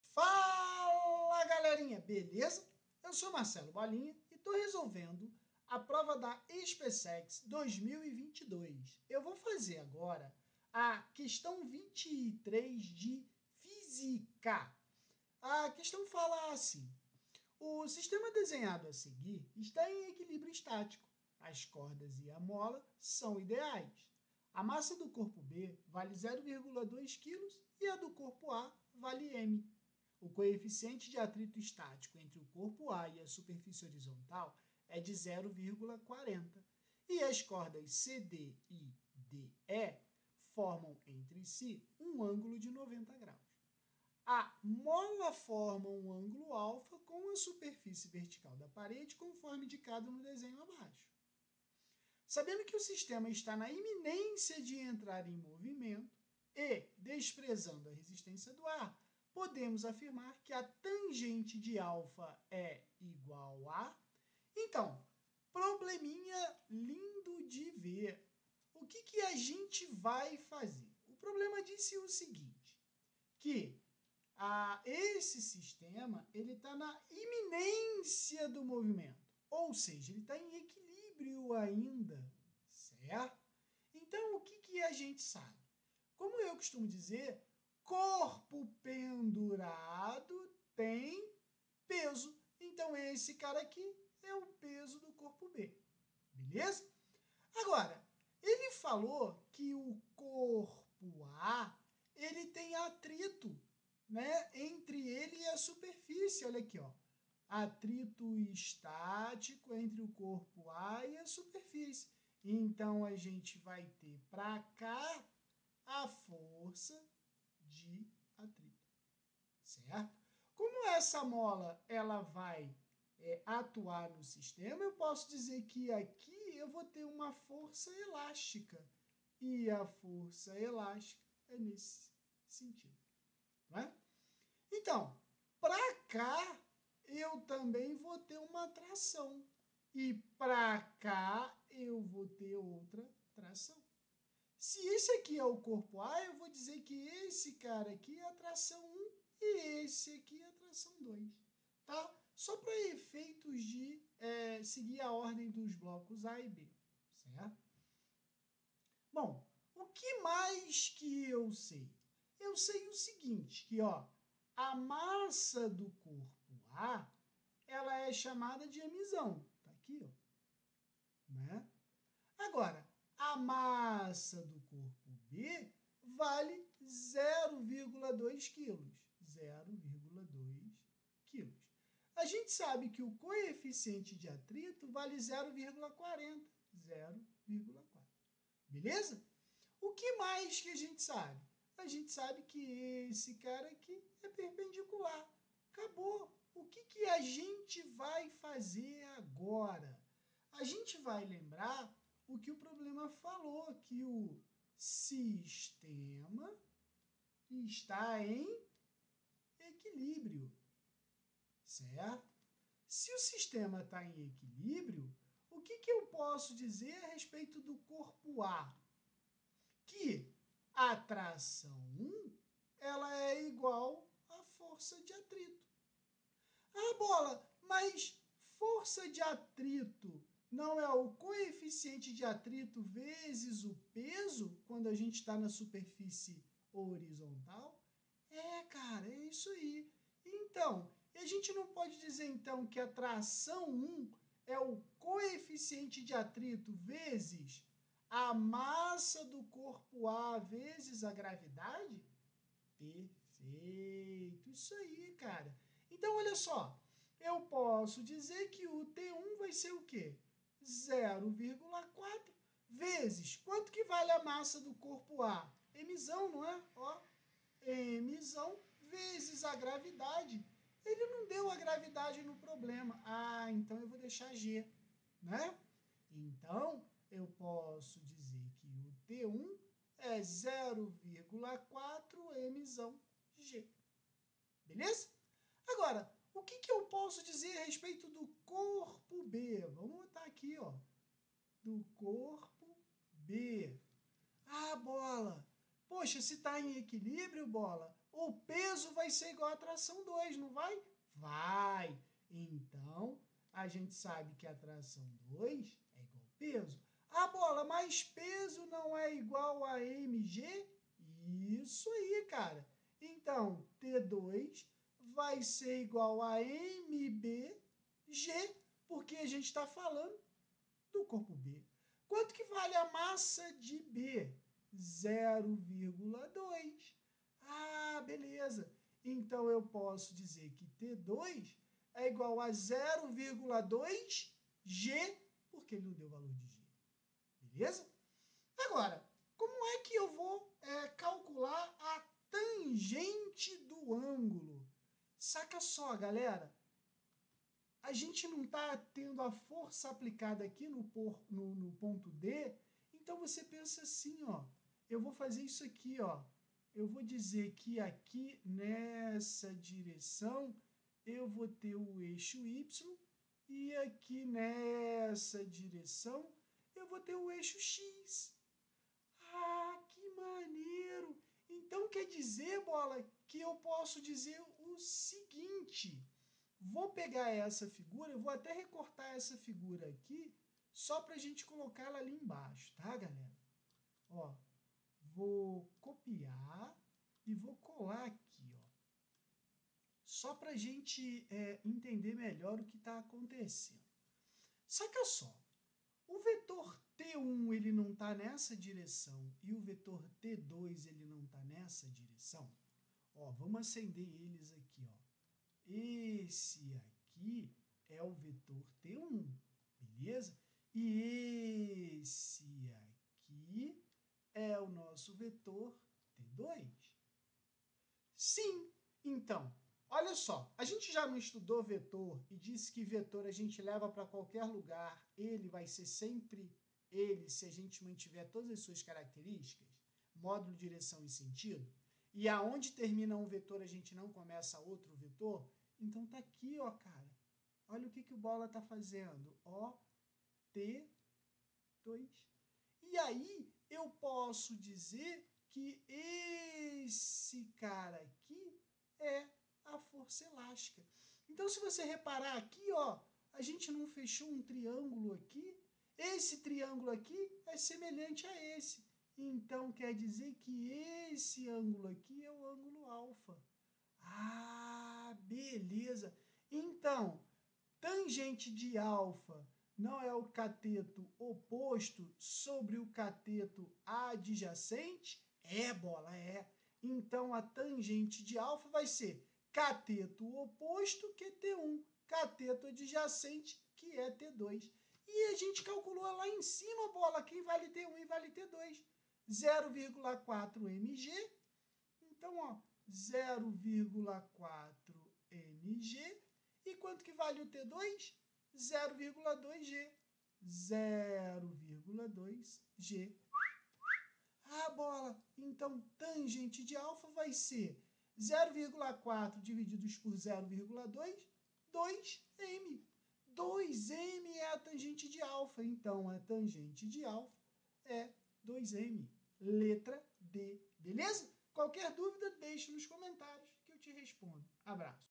Resolução da prova de FÍSICA – EsPCEx – 2022. Questão: 23
O sistema desenhado a seguir está em equilíbrio estático. As cordas e a mola são ideais, a massa do corpo B vale
0,20 kg, a massa do corpo A vale M, o coeficiente de atrito estático entre o corpo A e a superfície horizontal é de
0,40 e as cordas CD e DE formam, entre si, um ângulo de 90°. A mola forma um ângulo α com a superfície vertical
da parede conforme indicado no desenho abaixo. Sabendo que o sistema está na iminência de entrar em
movimento e desprezando a resistência do ar, podemos afirmar que a tangente de α é igual a: